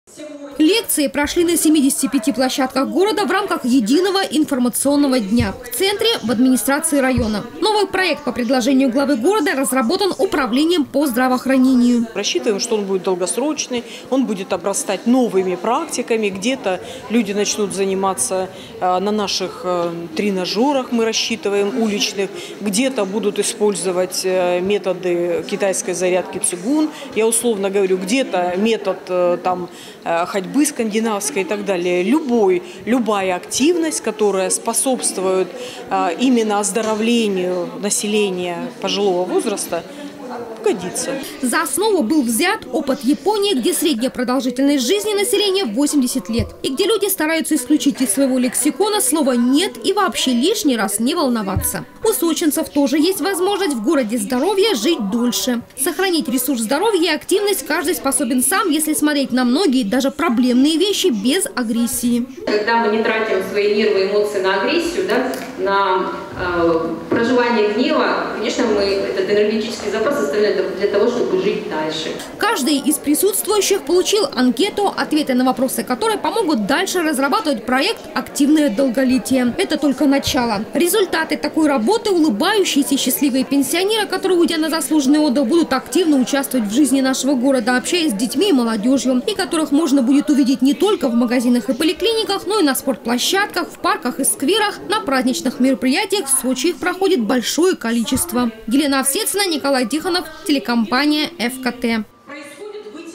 В Сочи пенсионеров просят жить дольше. Новости Сочи Эфкате
«Активное долголетие». Под таким названием в Сочи стартовал проект администрации города. Его главная задача — увлечь людей старшего поколения новыми возможностями сохранения своего здоровья. И это совсем не медикаментозная помощь, а позитивный настрой на жизнь.
Сочи. Новости.
Телеканал Эфкате.